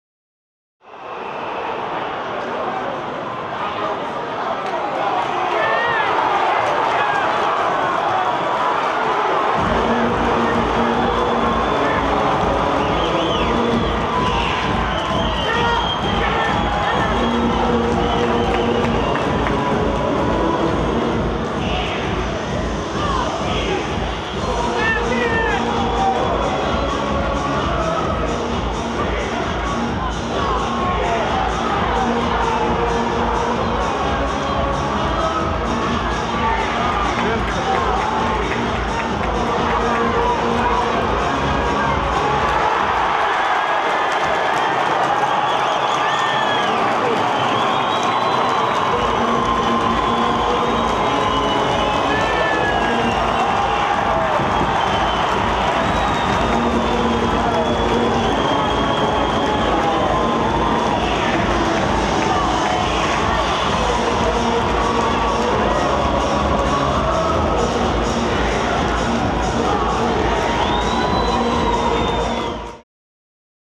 NICKERSON FIGHTS BERGIN PART II
Fife Flyers' Matt Nickerson has his second fight of the season with Braehead Clan's Kevin Bergin. Fife won the match 4-1 to qualify for the end of season play offs.